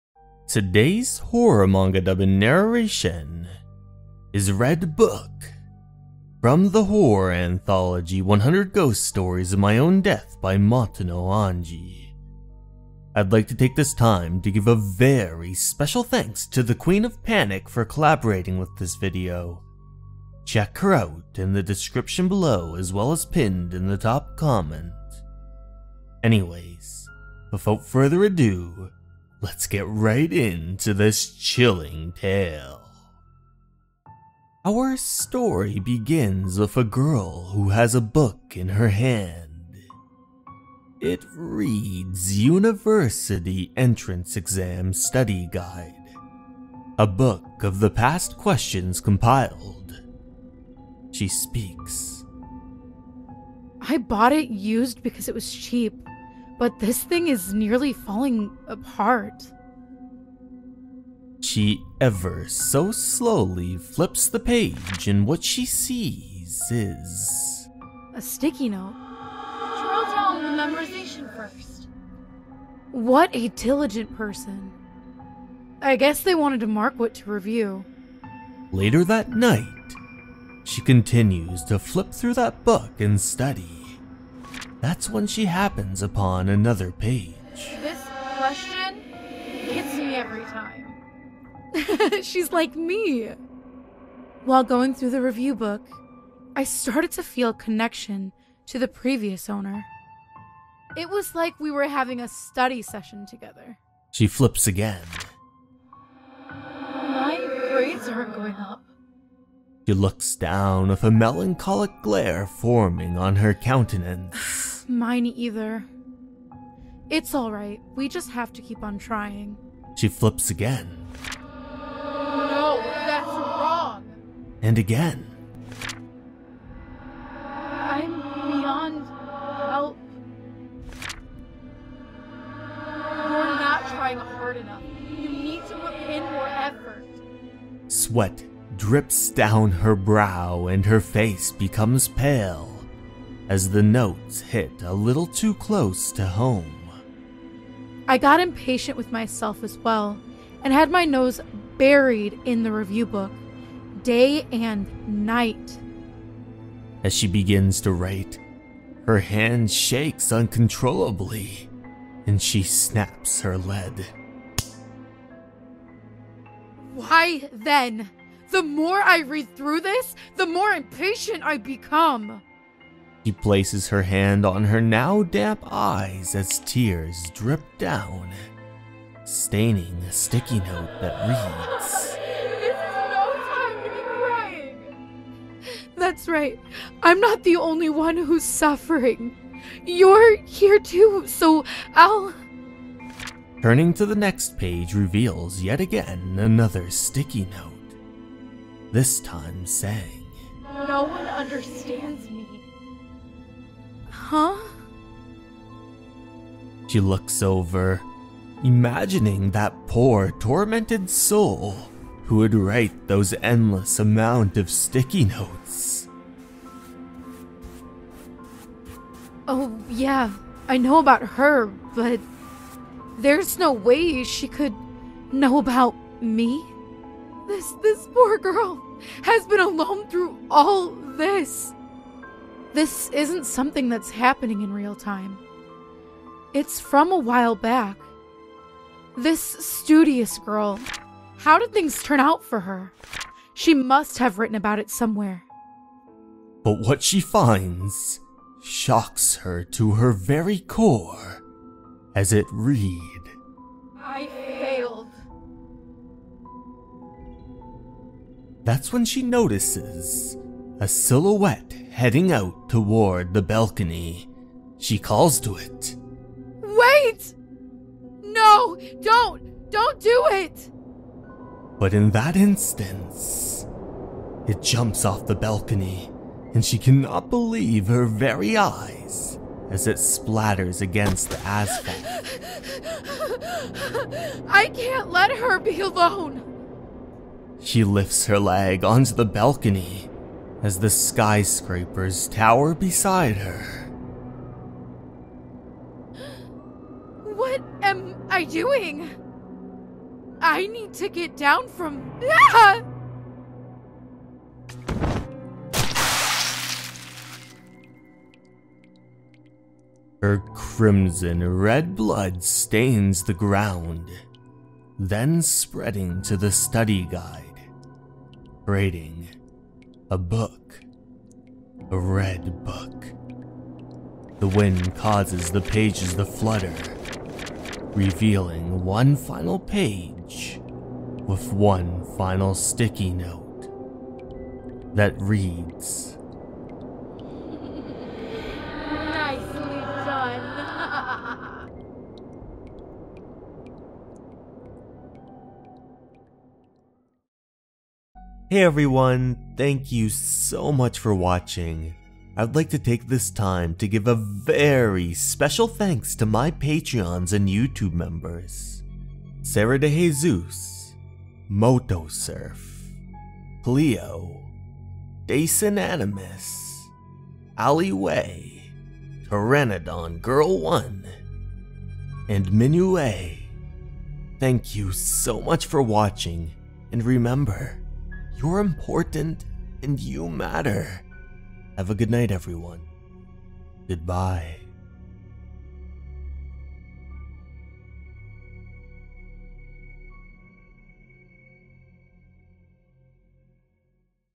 "Red Book" Animated Horror Manga Story Dub and Narration
Sit back, relax, and listen to this horror manga dub narration. This narration narrates horror Manga and scary story "Red Book"

For the best effect listen to this scary manga narration with the lights off in the dark.

Special Thanks To:
Matono Anji for their amazing story and art!
Myuu for his awesome music!
The Queen of Panic (@thepncwithin4949 )